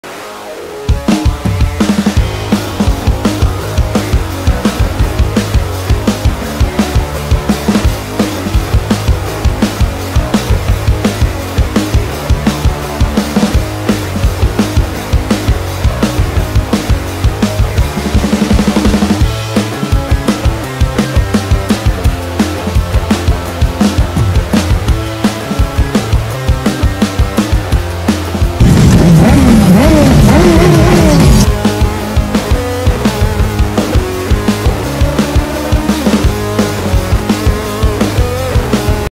Superbike Death Racing on Highway 😱 fu#king mad Death call !
Superbikes drag racing on highway 😱 fu#king mad close calls